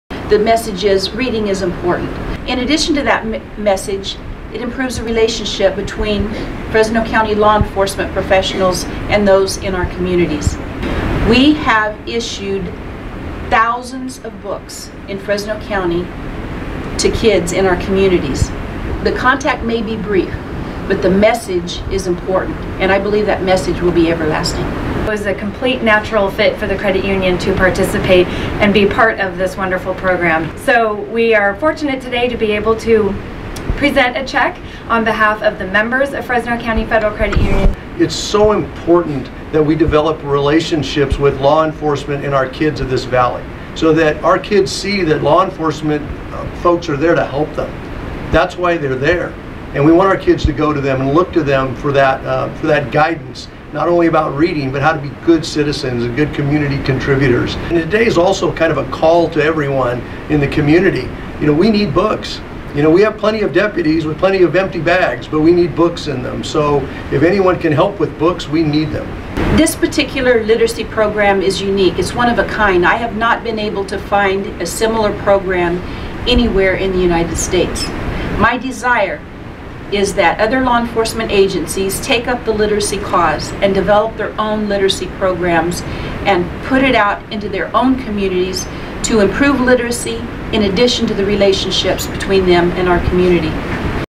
Fresno County Sheriff's Reading Posse Gets Financial Boost
Fresno County Federal Credit Union donated $2,300 to the program which aims to increase literacy in our youth and in turn decrease crime in our community.